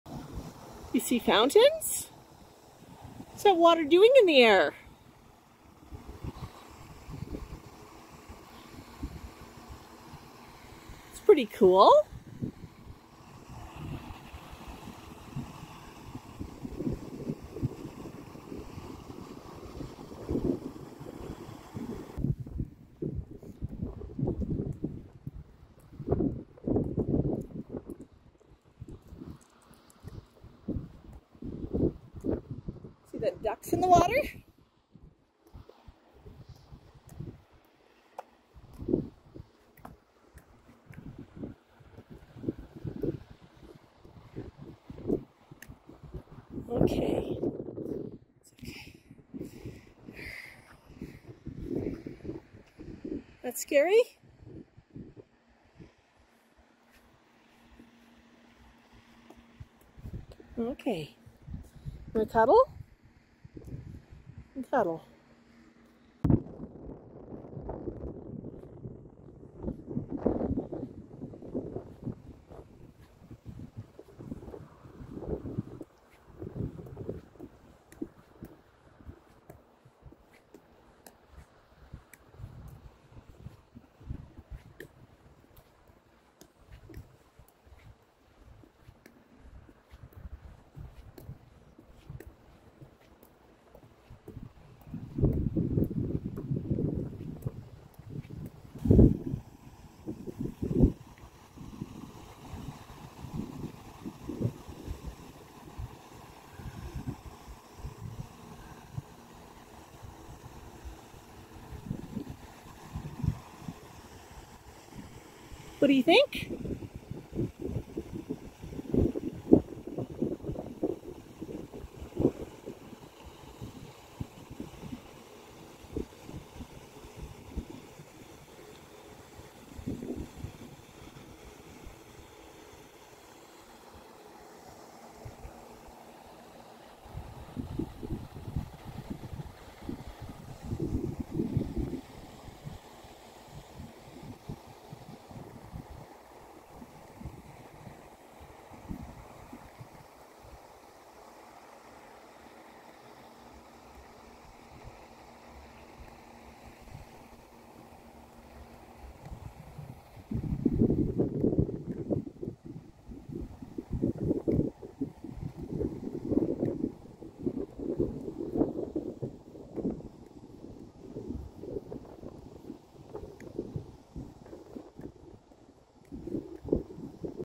Max Discovers Dancing Fountains
I see water dancing in the air!  Momager called the fountains.  I've never seen water do this before.  It was a very cool place to walk although I was a bit nervous about being on the edge of the city with all of the construction.

Have you ever seen dancing fountains like these before?  So cool!

Join the flock!  I want to tell you all about the great adventure of living with my pet hoomin.

Max Merch!  (T-shirts and Mugs)